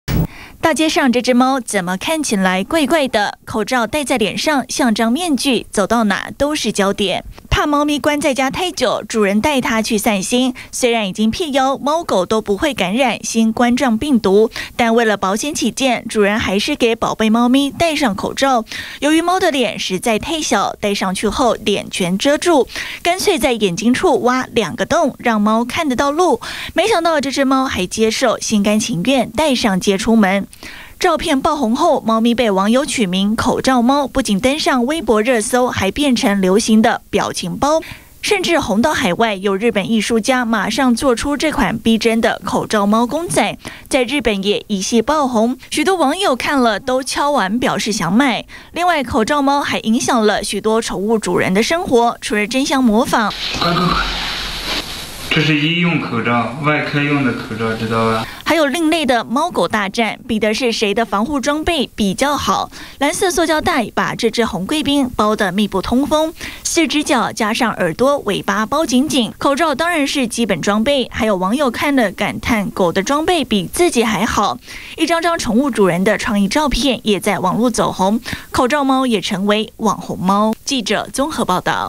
20200211中天新聞 陸「口罩貓」一夕暴紅 日本做逼真公仔
近日大陸受新冠肺炎疫情影響，民眾心情都很緊繃，有位主人帶貓出門散心，還幫牠戴上口罩，但因為貓的臉太小，整張臉被口罩遮住，主人乾脆把口罩當面具，在眼睛的地方戳兩個洞，照片還紅到海外，日本藝術家，還特別幫這隻「口罩貓」設計公仔。